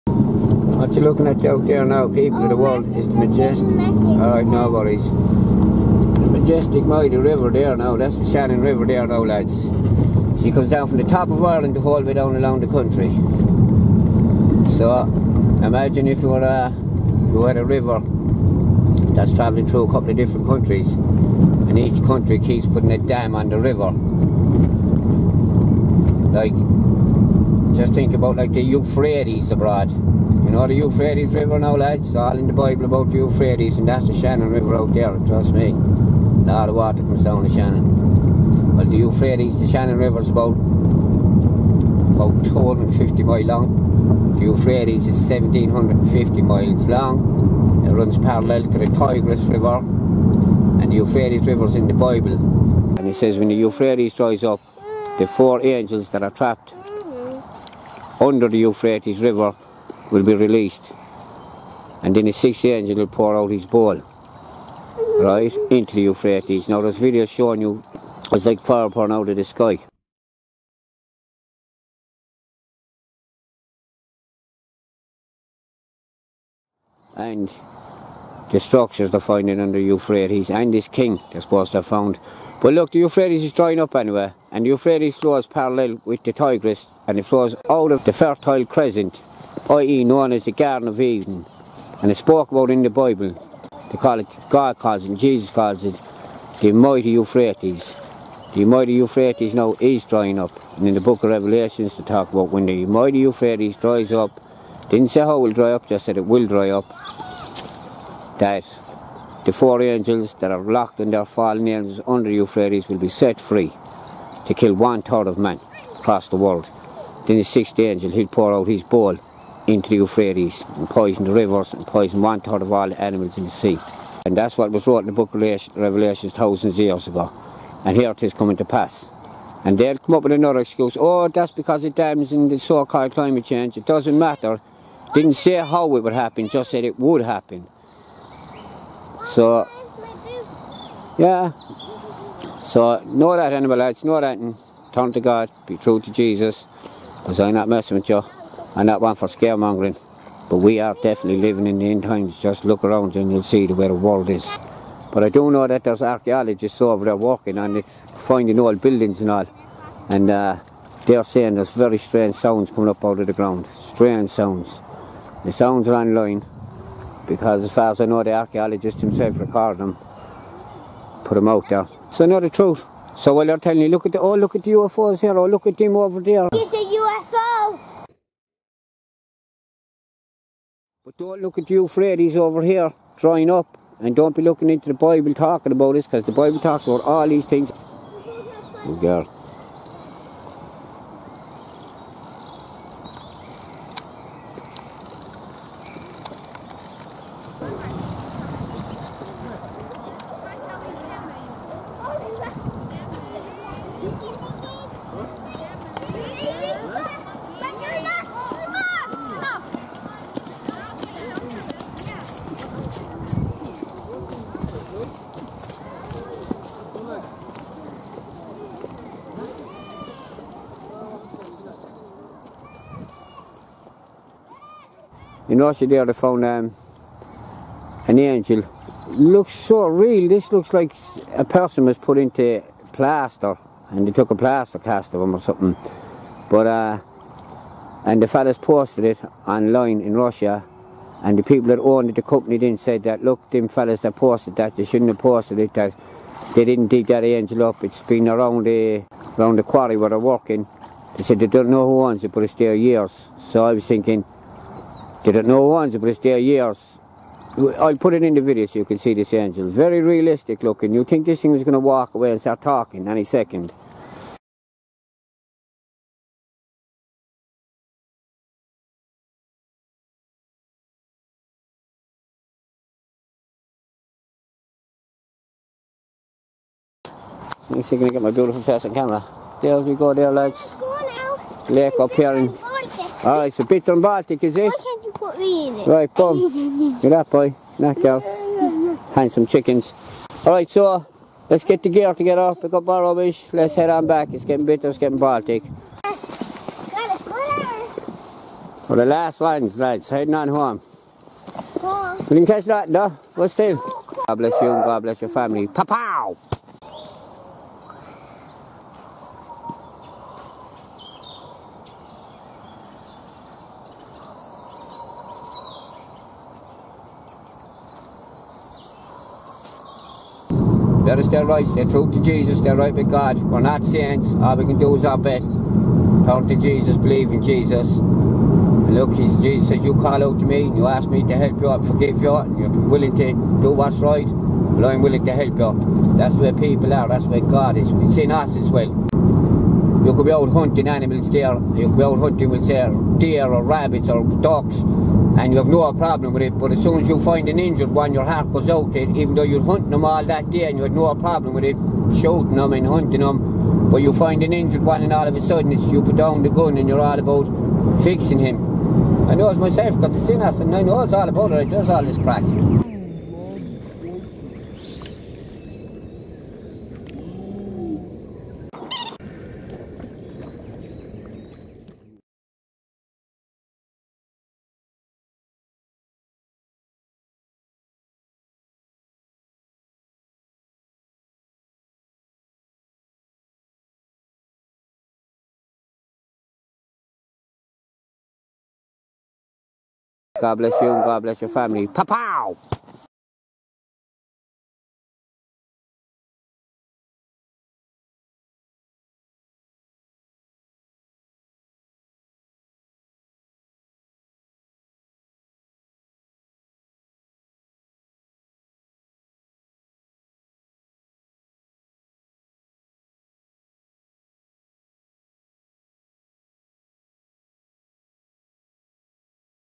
ANGRY ANGLER. LOOK A UFO BUT DONT LOOK AT EUPHRATES .
Euphrates is drying up and in the Book of Revelation it says the Euphrates will dry up. Didn't say how it would dry up, just said it will dry up. Then the 4 falling Angels will be released from under the Might Euphrates the six Angel will poor out his bowl.